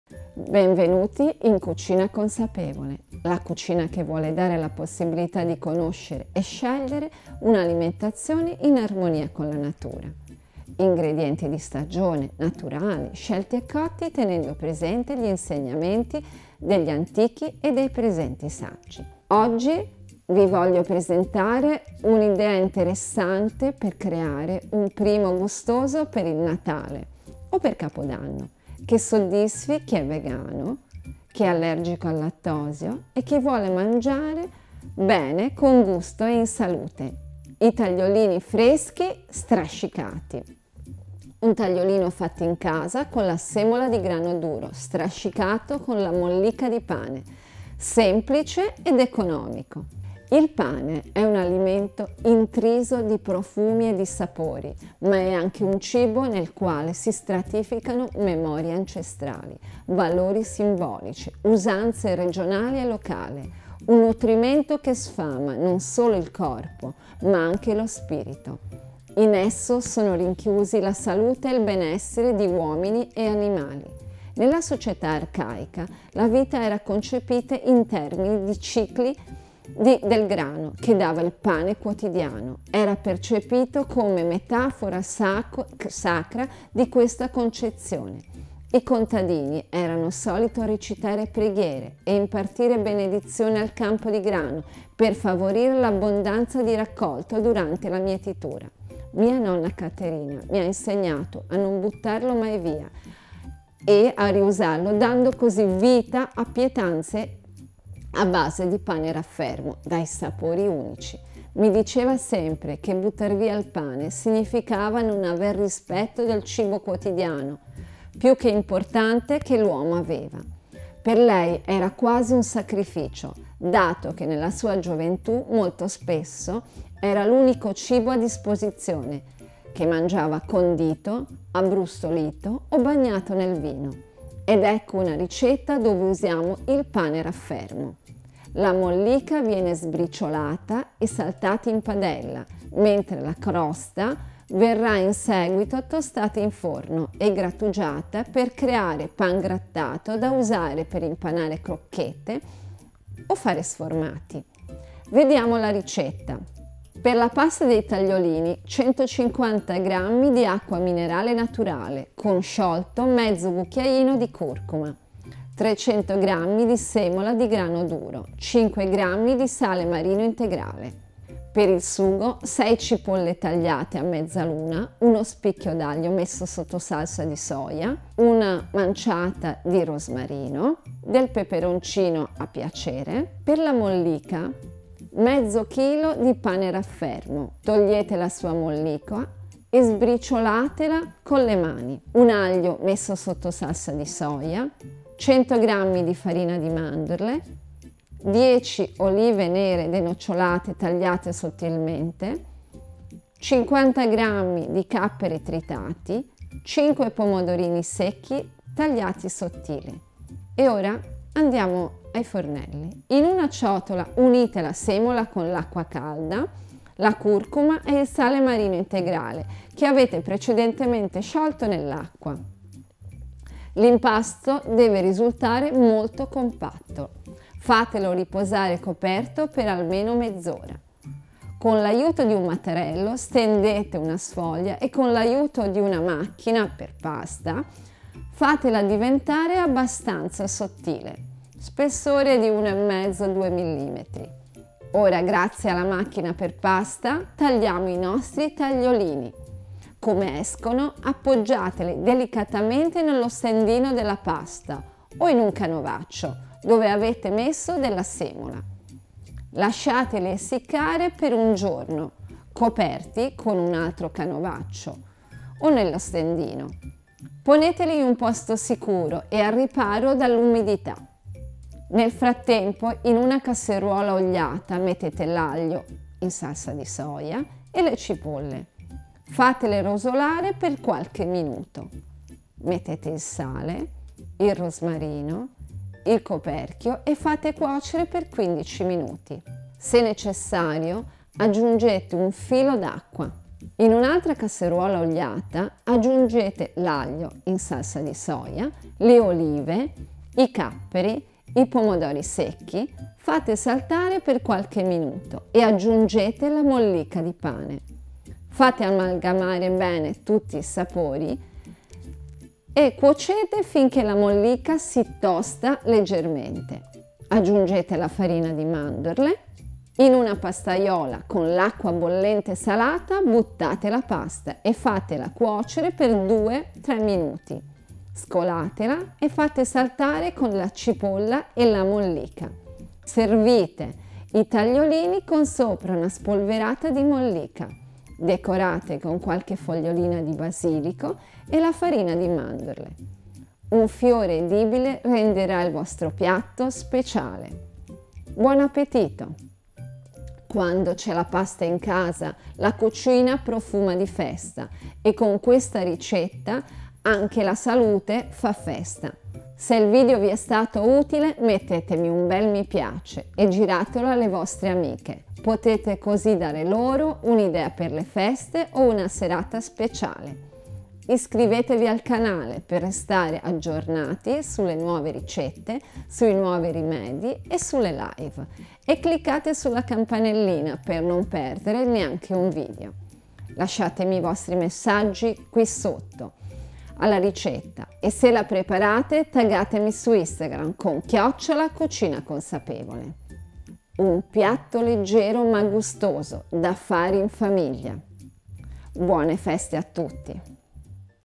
Tagliolini freschi strascicati
Oggi vi presento i tagliolini fatti in casa con la semola di grano duro, strascicati con la mollica di pane: un piatto semplice ed economico, che richiama La Sapienza delle nostre nonne.

+ Produzione:
Francesco Carrieri

+ Ristorante:
La Coccinella Natural Food, Via dell'Eremo 8, 57127, Livorno.